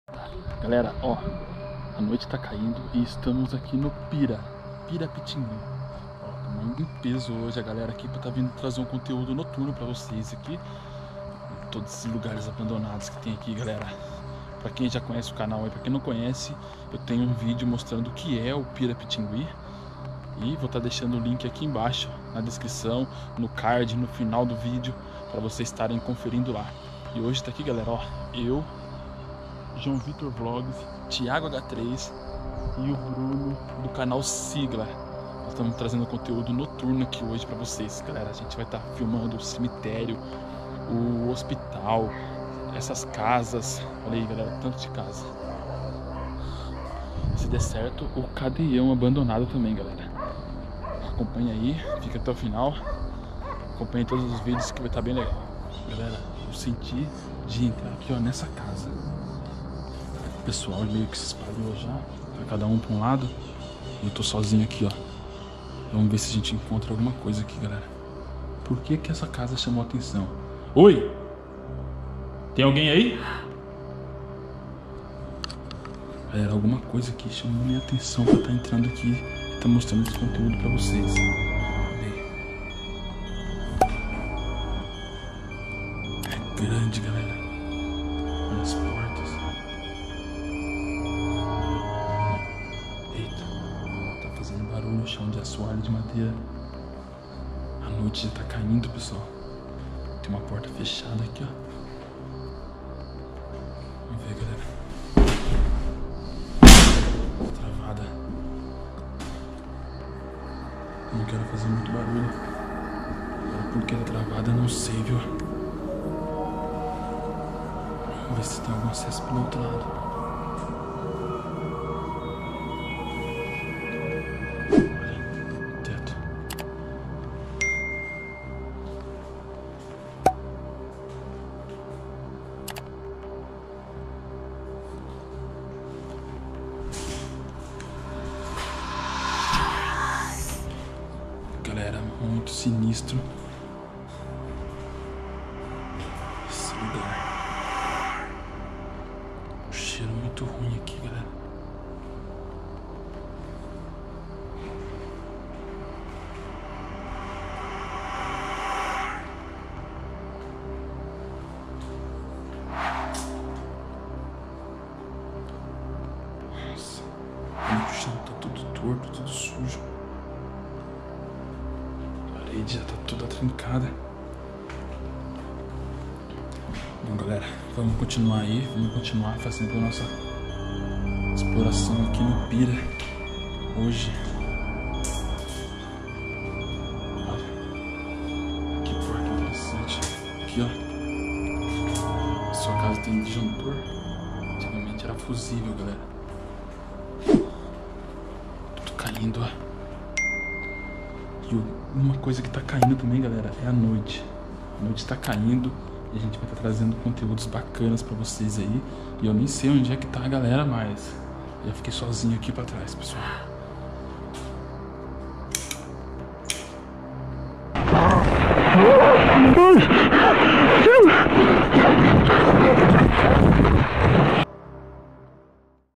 Cidade Fantasma a noite. Parte 1
Esse vídeo é o primeiro de uma série de vídeos noturnos na cidade fantasma (Pirapitingui) O maior Leprosário do Brasil.

Obrigado 🙏

Mais vídeos sobre o local